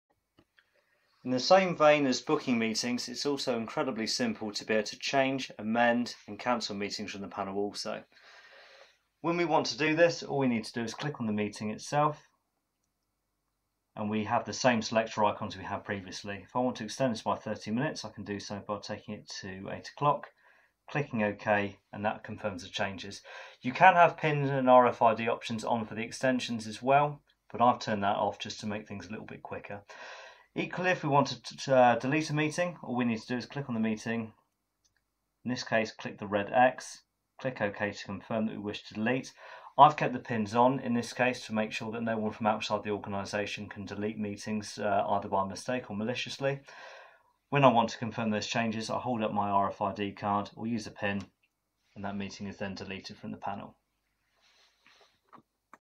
Evoko Liso - Extending and deleting meetings
How to amend, delete, or end early a meeting from the Evoko Liso main screen.